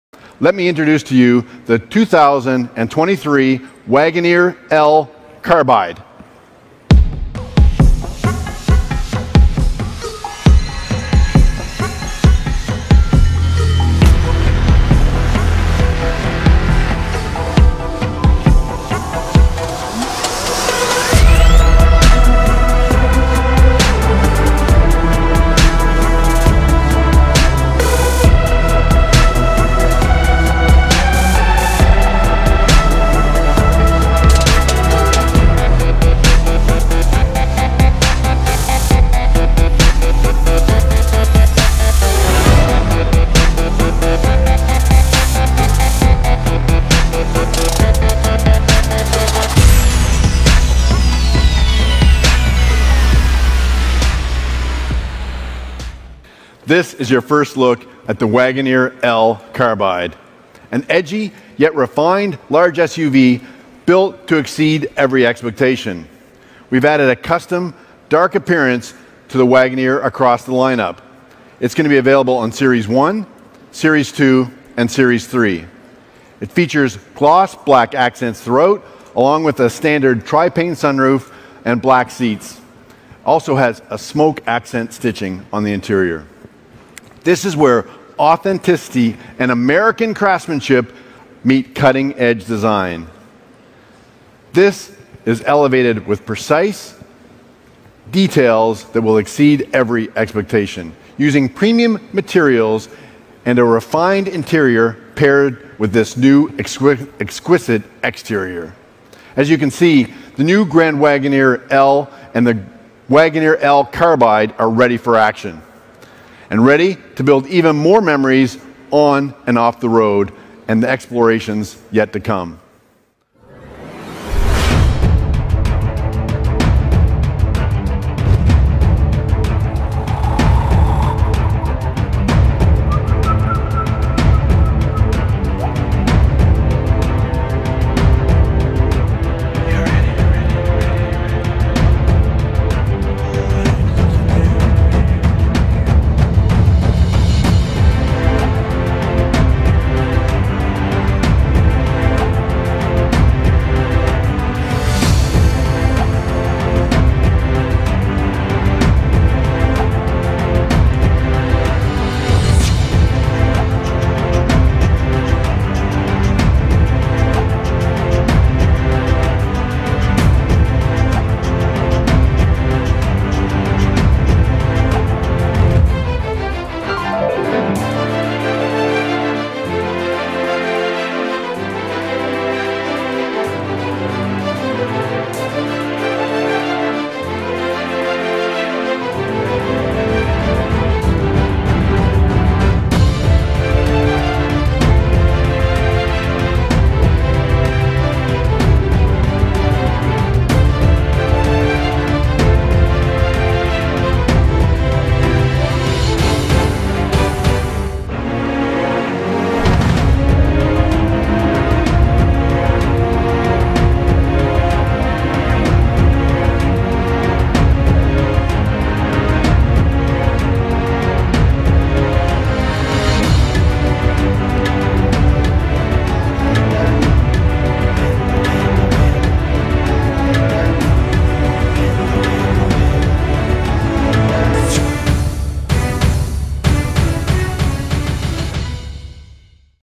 FIRST LOOK NEW 2023 JEEP WAGONEER L CARBIDE | DARK EDITION HUGE LUXURIOUS SUV | INTERIOR | EXTERIOR
DETAILS ABOUT THE CAR:
Jeep revealed its long wheelbase versions of the Wagoneer and Grand Wagoneer at the New York Auto Show, and somehow they’re even bigger than their rivals from GM and Ford. Icing on the cake from Stellantis during the show comes in the form of a new  2023 Jeep Wagoneer Carbide trim that makes luxury look more aggressive than ever before. Checkout the 2023 Jeep Wagoneer L Carbide edition in this reveal video which consists its interior, exterior design, specs, engine, safety and other important details.

Carbide, in chemistry, is the name for a compound consisting of Carbon and a metal. The industrial uses can be found in top-shelf drill bits, they’re more expensive but also feature longer life spans and superior durability. When it comes to the Jeep Wagoneer Carbide, the color resembles that of elemental Carbon, a grayish black hue, that is accentuated both on the exterior as well as the interior of the vehicle, similar to the Obsidian package offered on the Grand Wagoneer.

The Wagoneer Carbide rides on Gloss Black wheels and features a panoramic triple-pane sunroof as well as Gloss Black grill rings, roof rack, and badges. Lower front and rear fascia’s as well as door flares sport a Diamond black color. Also in Gloss Black are the daylight opening moldings, headlamps, mirror and sill cladding appliqués. A similar dark theme is found inside. The Carbide package comes with a new hydrographic dash trim and a Smoke color accents the Global Black seats, instrument panel and doors.

The new trim will be available sometime this spring for the 2023 Jeep Wagoneer, and the second half of 2022 for the new Wagoneer L. At the time of this writing it isn’t available on the build and price tool yet, however, it is expected to be more affordable than the six-figure Grand Wagoneer Obsidian. The 2023 Wagoneer L will feature a towing capacity of 10,000 pounds and feature the Standard Output (SO) Hurricane engine supplying 420 horsepower and 468 lb-ft of torque.

A distinct blacked-out appearance will be available only on Wagoneer models as part of the new Carbide package. The kit coats the wheels, grille elements, mirrors, and more in gloss black, and the interior features black seats and special trim on the dashboard. It'll be available on the regular 2022 Wagoneer this spring and join the 2023 Wagoneer L lineup when it goes on sale in the second half of this year. The 2023 Grand Wagoneer will also hit showrooms at that time, but Jeep hasn't yet announced pricing for either of the new long-wheelbase models.